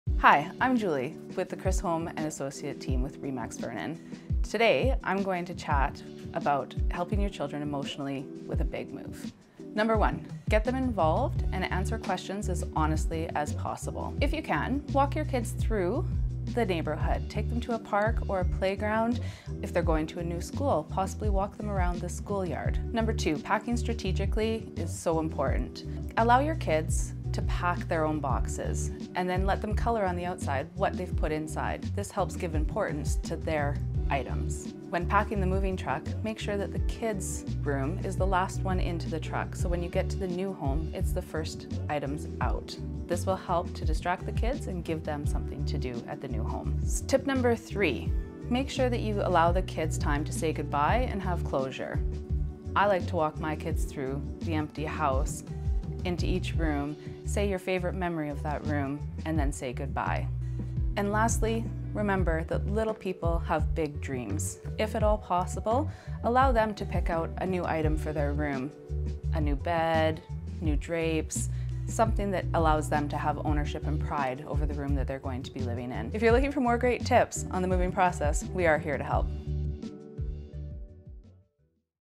Tip Tuesday - Tips for Moving with Kids
Realtor Julie Lutz is here to give you her tips and tricks on how to make the moving process go smoothly with kids.

Want to see more Enderby BC Real Estate, Armstrong BC Real Estate, and Vernon BC Real Estate?

Chris Holm
Re/Max Vernon